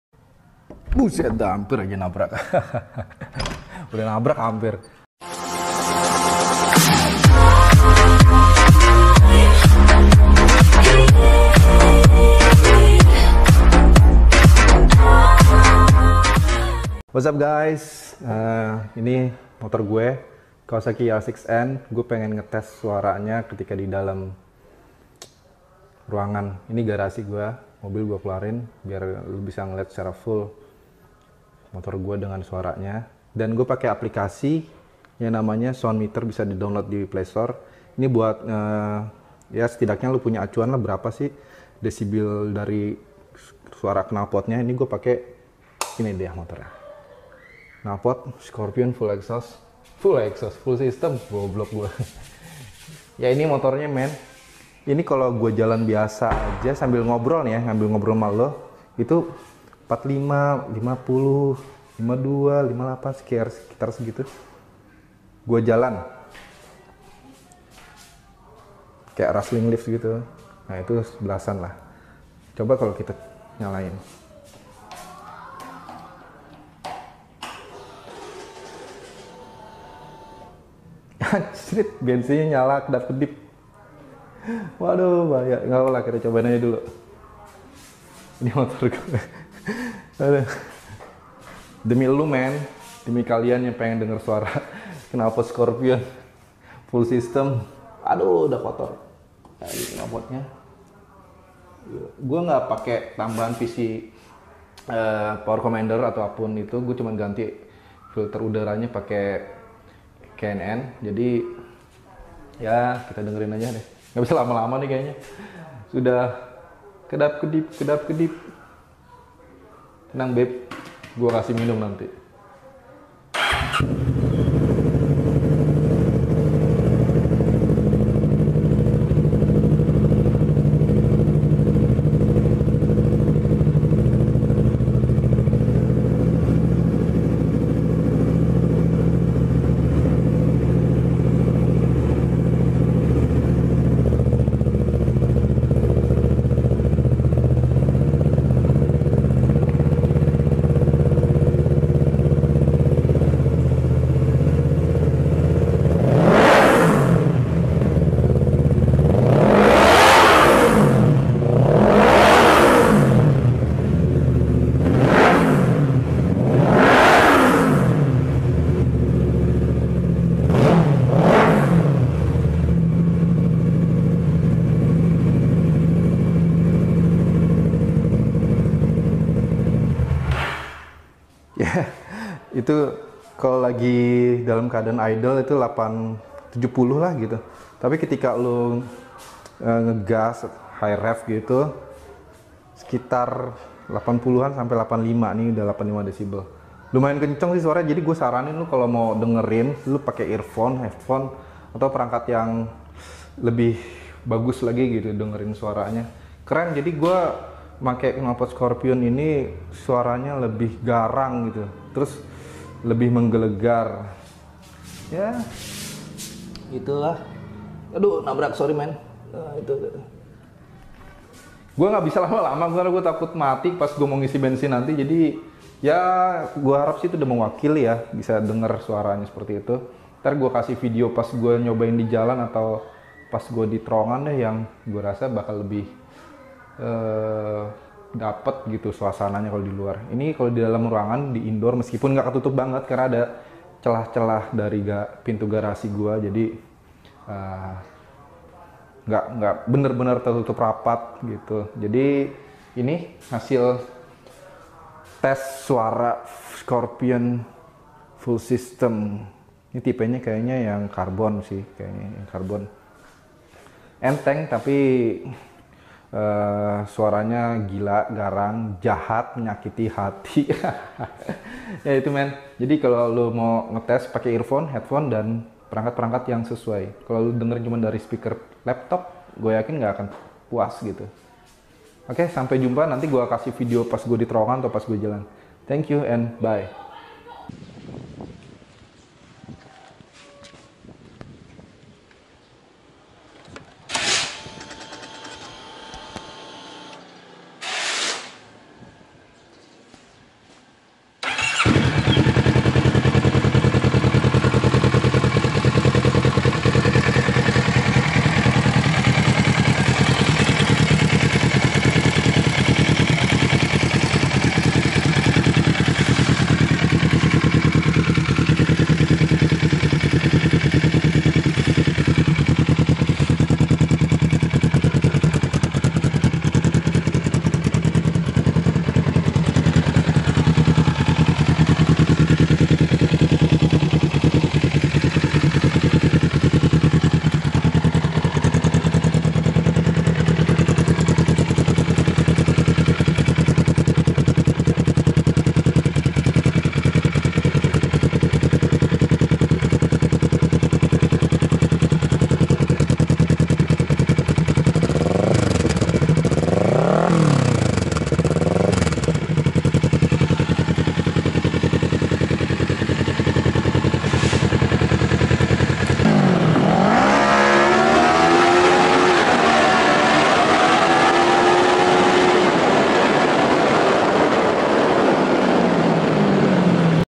Kawasaki ER6N - Review Suara Knalpot Scorpion Serket Titanium Indoor dan Terowongan
Kawasaki ER6n pake knalpot Scorpion carbon, fullsystem (dBkiller dilepas) tanpa piggybag. Pengukuran tingkat kebisingan pake Soundmeter. Pemakaian cukup lama, sekitar 4 tahunan. Suara menggelegar, di dalam dan luar ruangan. Air filter diganti K&N. Sori gasnya kurang poll, gas tipis aja krn bensin lagi tiris..

Please watch it in Full HD..